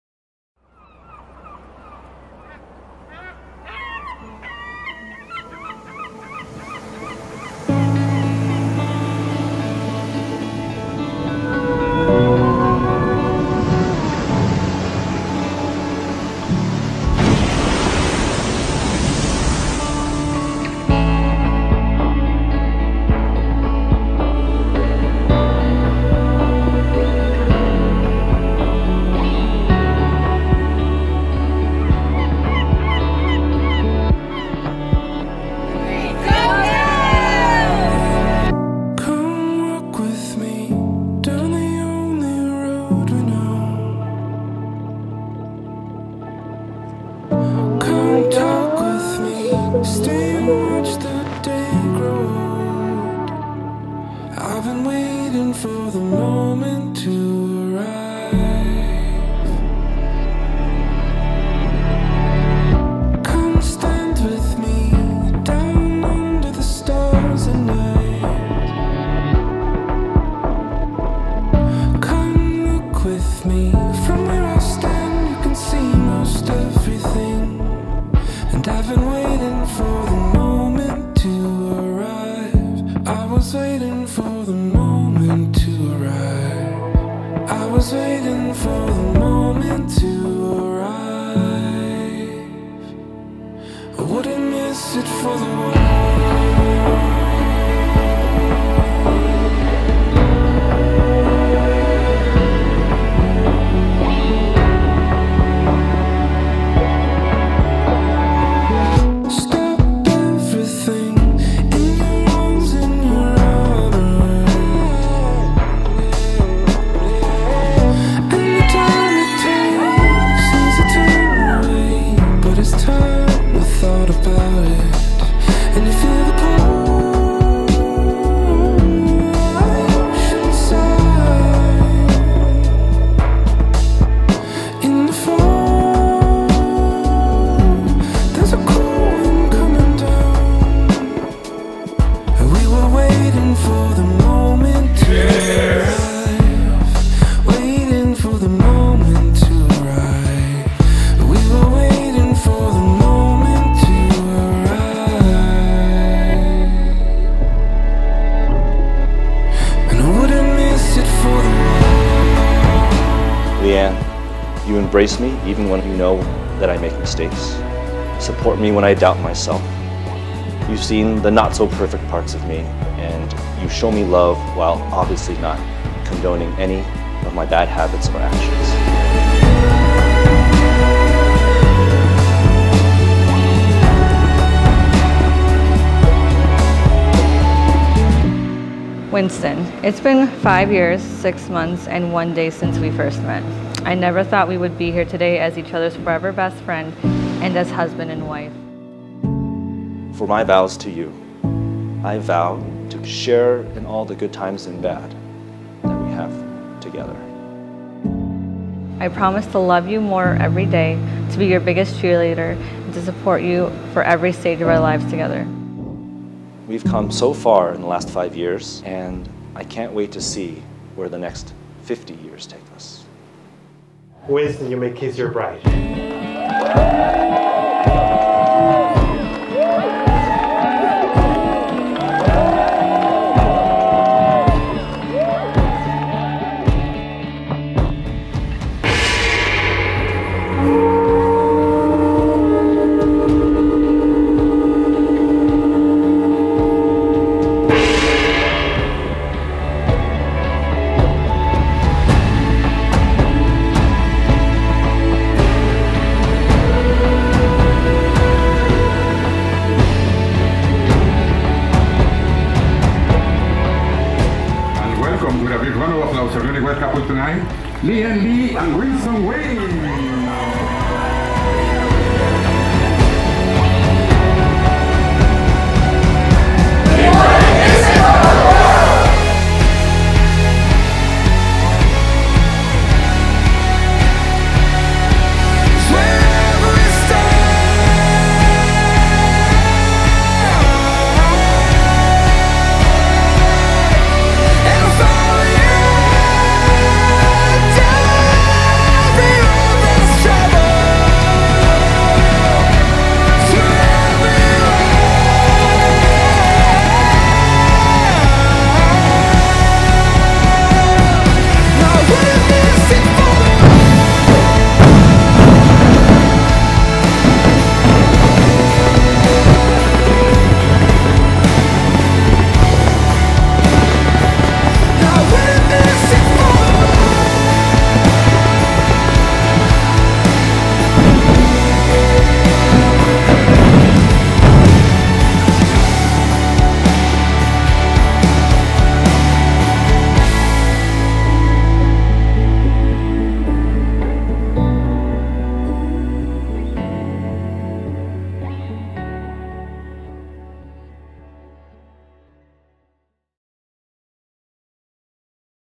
How amazing the ENDING is !!?!! Surprise Wedding Same Day Edit in Cabo Mexico Sunset Monalisa
We aim to astonish the couple by presenting a same-day edit encapsulating all the extraordinary moments of their special day. Families and friends have gathered from around the globe, and we believe it would be a heartfelt experience for them to relish these remarkable moments alongside the couple. Pouring our hearts and souls into crafting this film, we sincerely hope that you find joy in every frame until the very last second. Peace out, onto the next celebration

Congrats to our beautiful couple LeeAnn & Winston
Photography: tinphotography
Cinematography: the ZENIT way